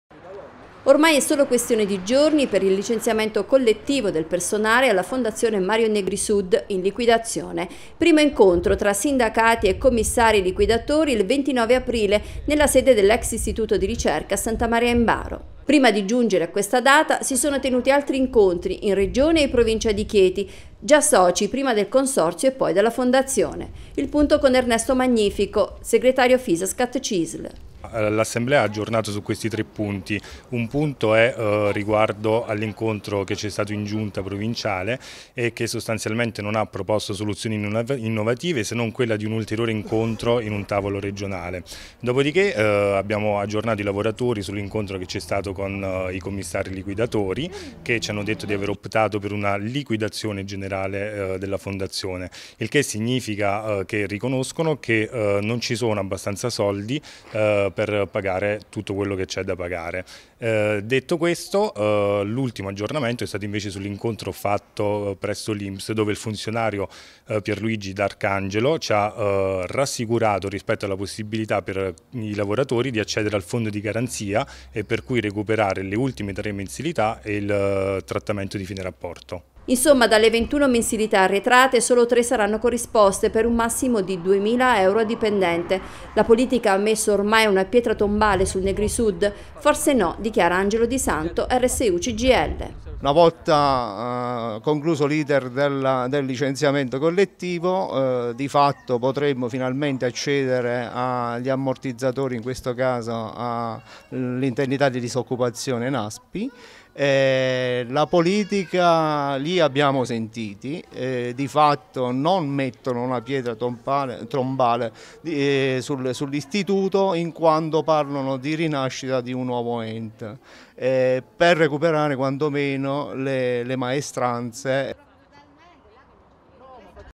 Negri sud, licenziamento collettivo a giorni
Lanciano, servizio Tgmax 23 aprile 2015. Ricerca: entro il 7 maggio i dipendenti della Fondazione Negri Sud di Santa Maria Imbaro saranno effettivamente licenziati, riceveranno solo 3 mensilità delle 21 arretrate e il Tfr. La politica ha messo una pietra tombale sull’ex consorzio? Forse no.